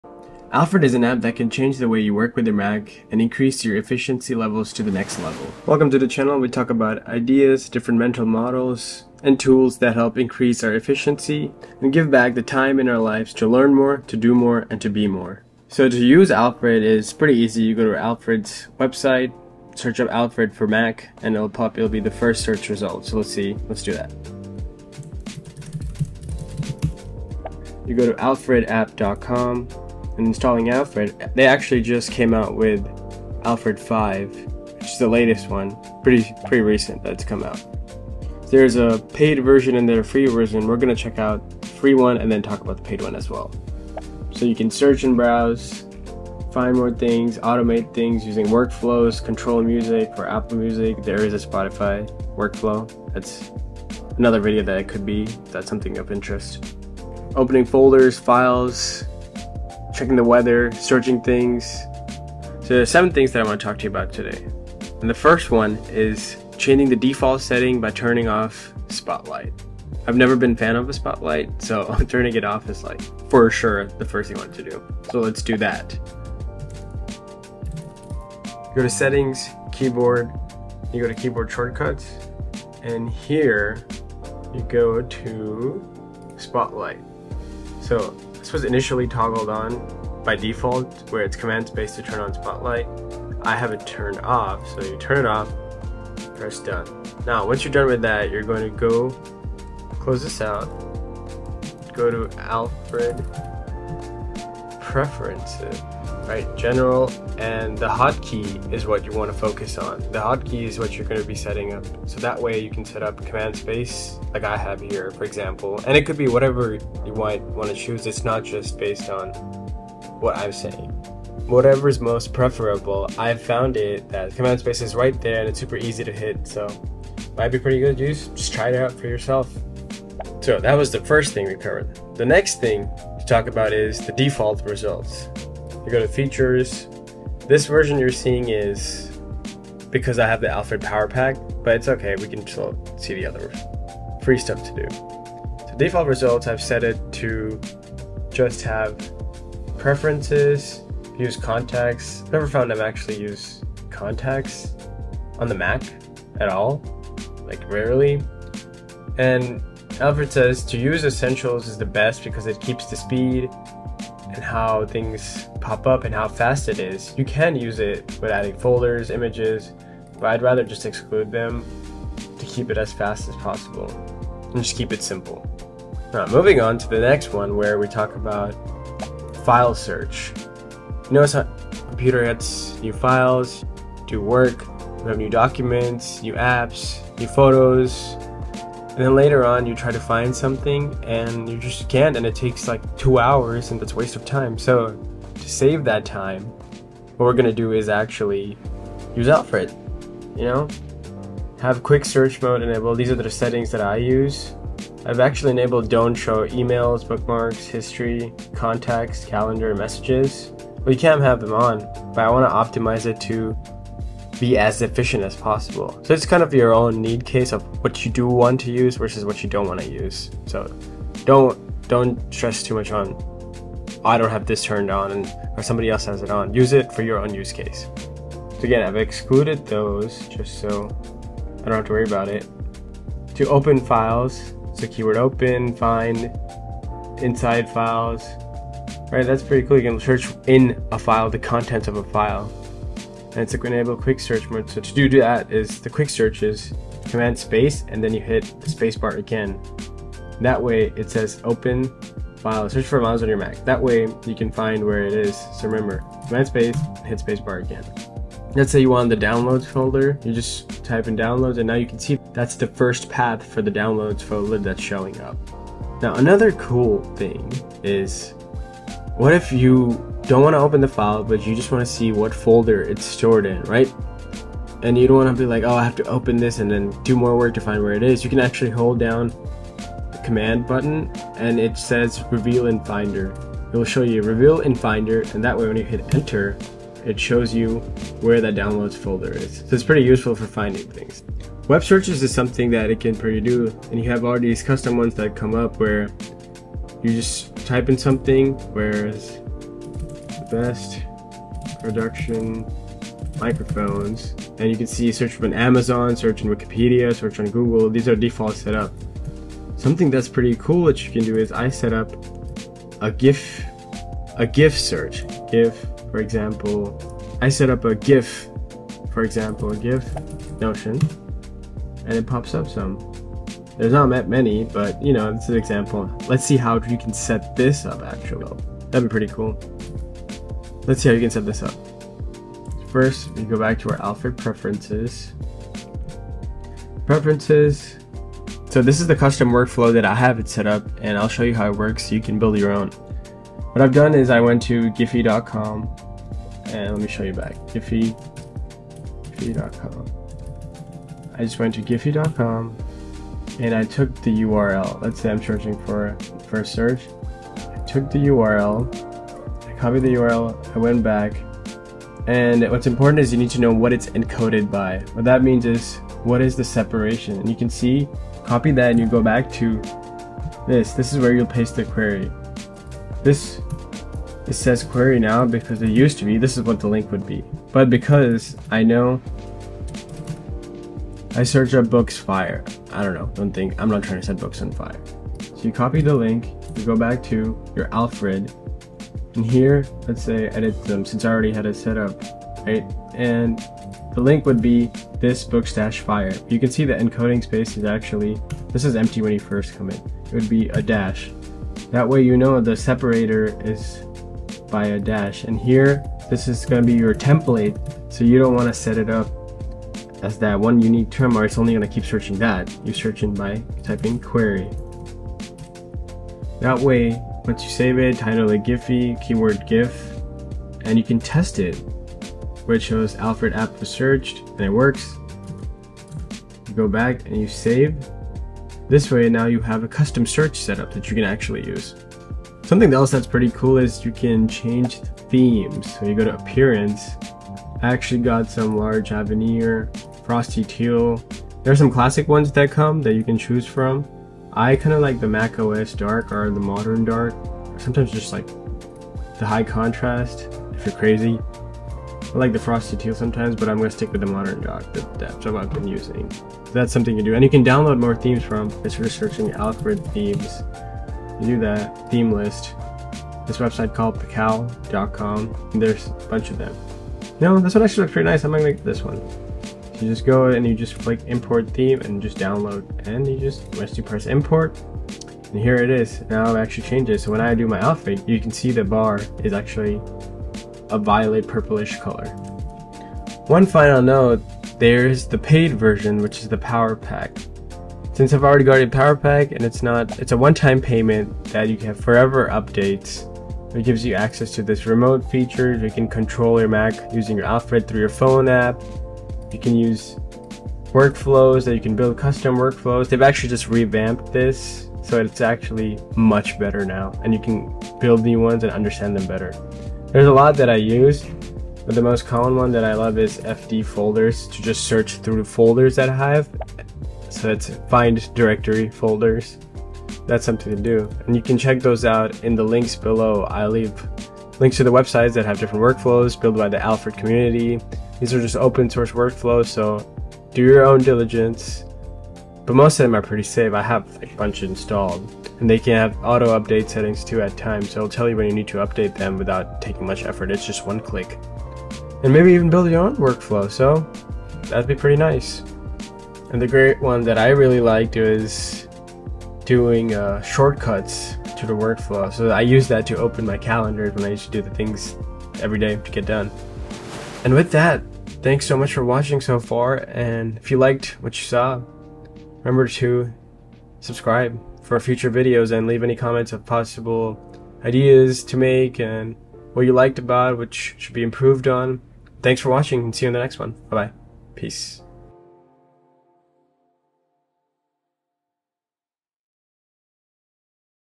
How to Increase Your MacBook Efficiency with Alfred 5 | Quick Setup (FREE)
The most essential app for your macbook to save you time is Alfred. Learn to use in under 10 minutes and save months of time.

Workflows

- Weather workflow
- Spotify mini player
- Reminders Workflow
- Control Volume and Brightness
- Search in Omnifocus
- Search Apple Notes
- Emoji Search
- Generate Markdown Table
- Time Zones
- Movie and TV Search
- Go Incognito
- Mac awake
- Temp Email
- Down media

Learn new skills and start creating content via Skillshare